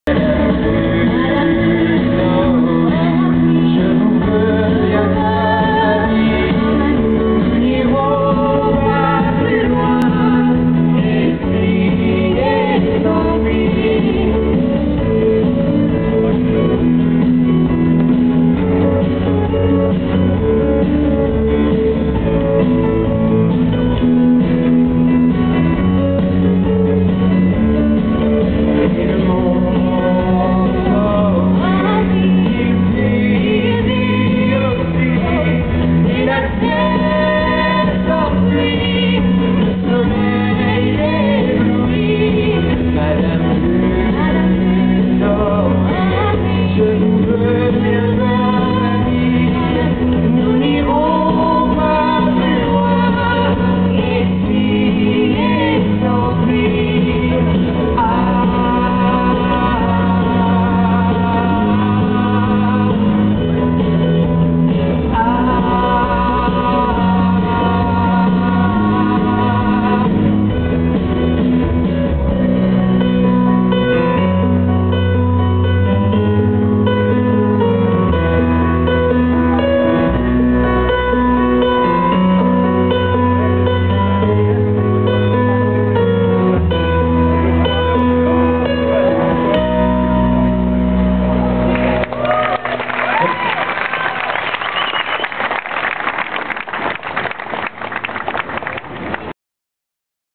Fête de la Musique 2011 à Strasbourg_GUIOM B
le groupe pop-rock GUIOM B sur la scène "Tête d'affiche" Place Kléber à Strasbourg.
Jeunes talents locaux vainqueurs de la scène "Tremplin 2010"